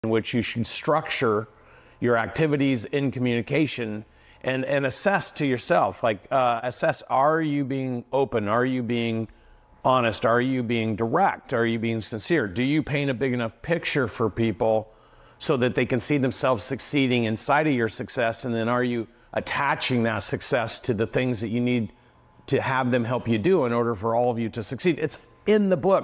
16 Laws of Communication by John Maxwell with Dawson
Maxwell's laws for effective communication and business success.